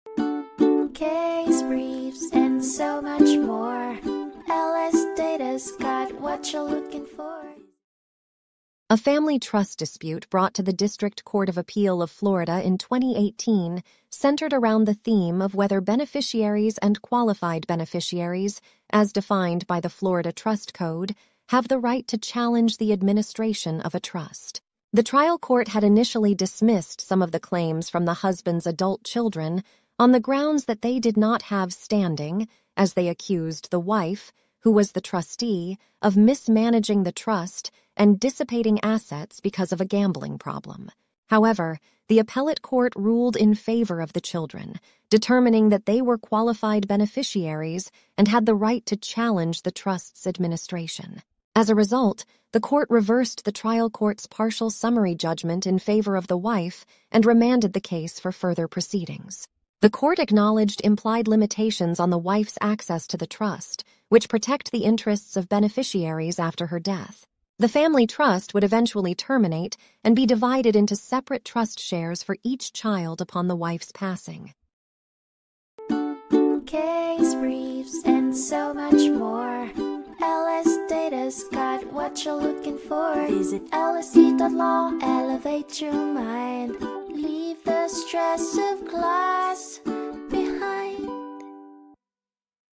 Rachins v. Minassian (2018) Overview | LSData Case Brief Video Summary
The case involves a dispute over the administration of a Family Trust created by a Restatement of Trust. The husband intended for the trust to become irrevocable upon his death, and the wife was the trustee with the power to distribute trust assets to herself for her health, education, and maintenance. The adult children of the decedent claimed they had standing to challenge the wife's administration of the trust, and they alleged that she dissipated trust assets due to a gambling problem. The trial court initially dismissed some of their claims for lack of standing, but the appellate court later determined that the children were qualified beneficiaries of the trust and had standing to challenge the administration of the Family Trust.

Rachins v. Minassian (2018)
District Court of Appeal of Florida, Fourth District
251 So. 3d 919